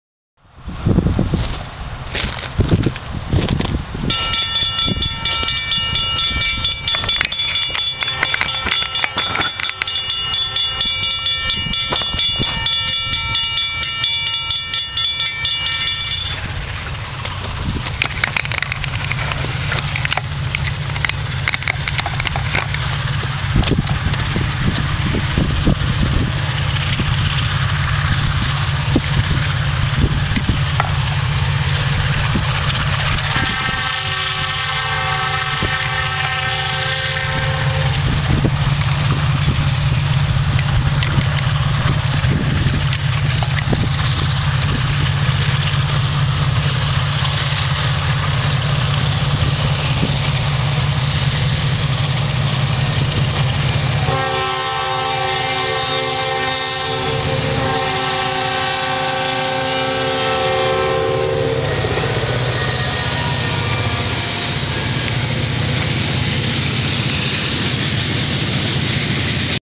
Chasing EJ&E 674 East - New Lenox, IL
EJ&E SD38-2 674 and one of EJ&E's three leased HLCX engines lead a eastbound manifest through New Lenox on the EJ&E Eastern Subdivision. Nice hybrid crossing bell! This train really puts on a show! Sorry about how crooked the camera is.